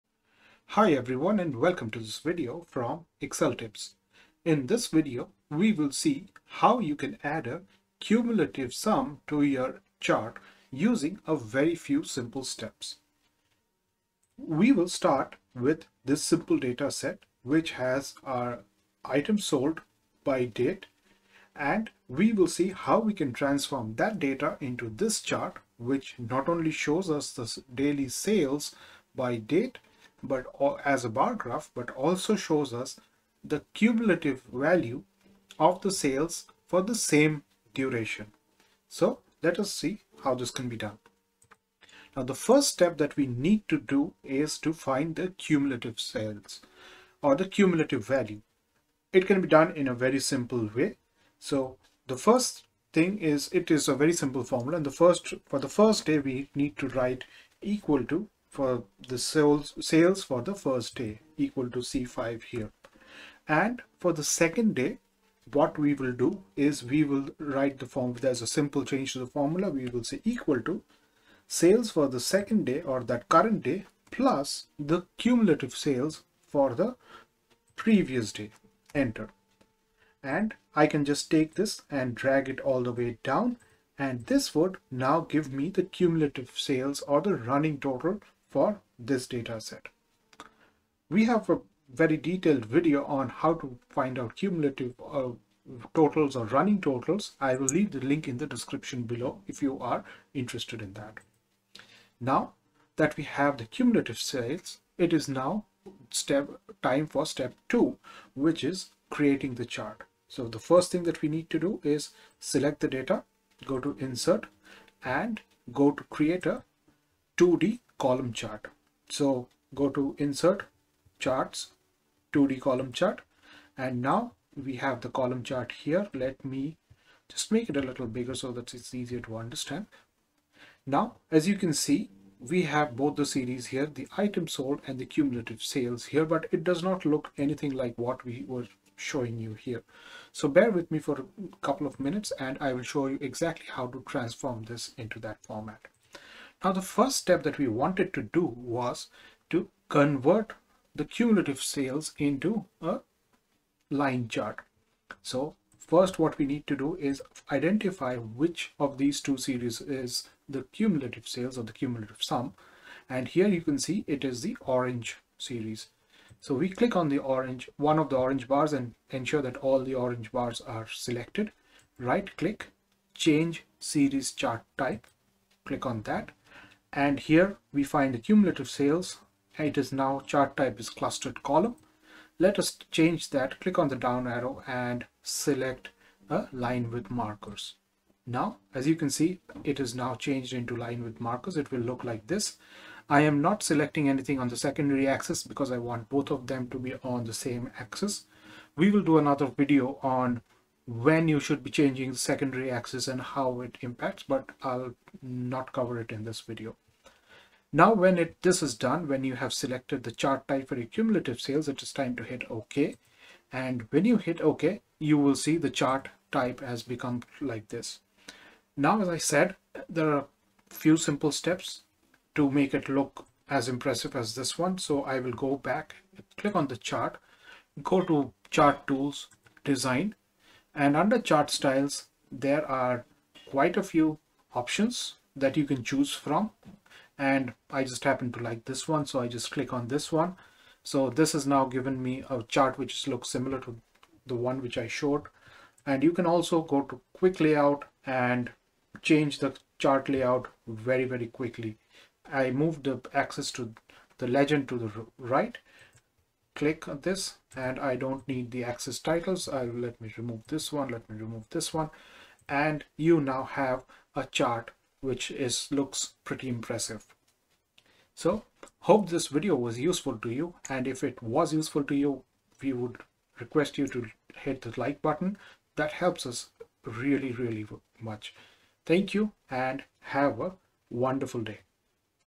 How to add cumulative sum to your chart in Excel
In this video, we will see how to add a cumulative sum to your chart - using simple steps

If you want to find out more about how to find the cumulative sum - do check out our video on finding the running total by clicking